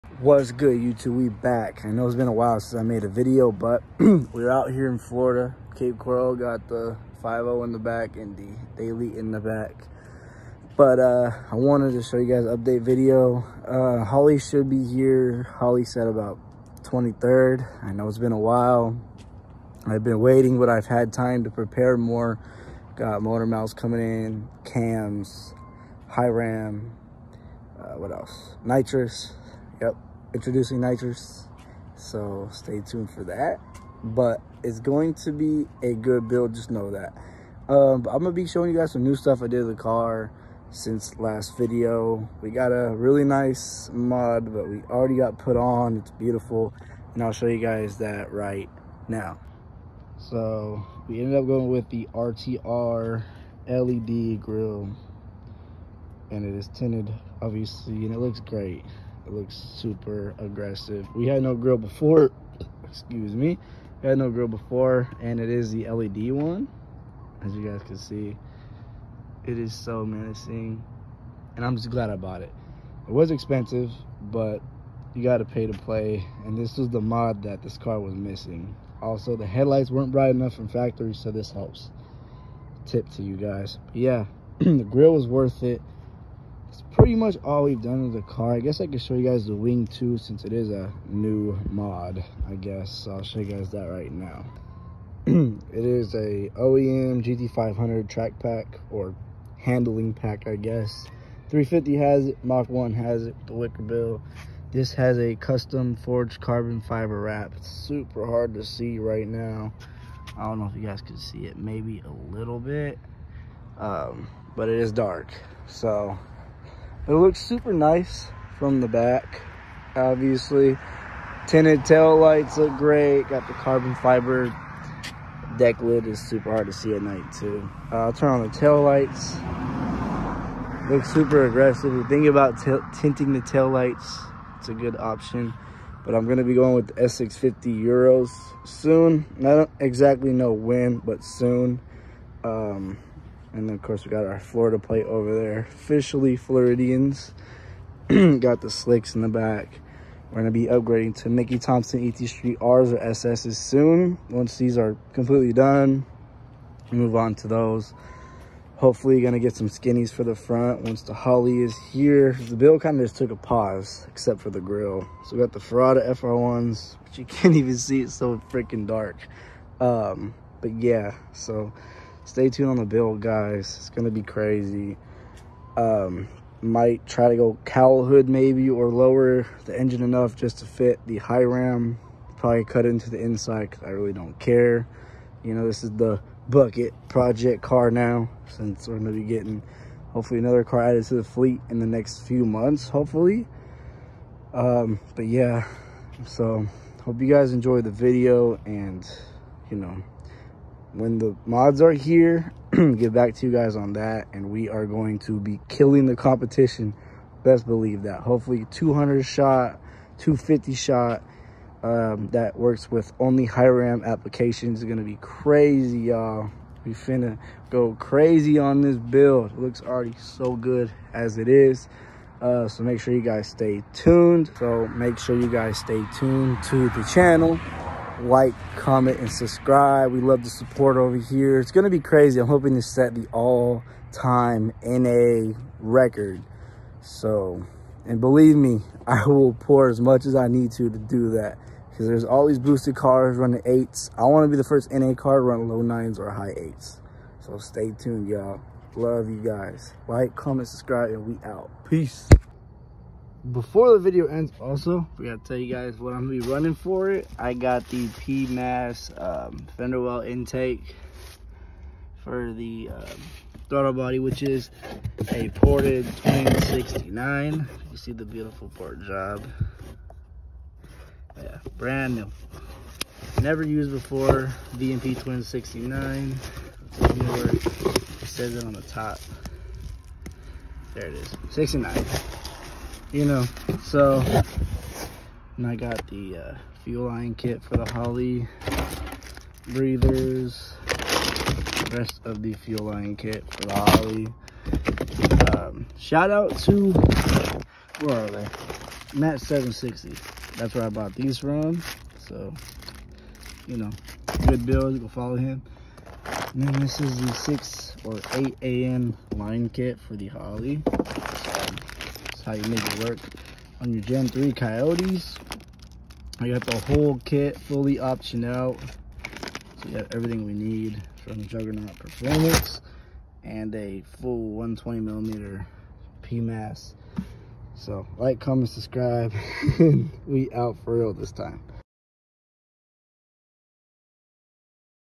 Mustang rtr led grille installed / update on the Holley build
Holley will be here estimated ship date feb 23rd!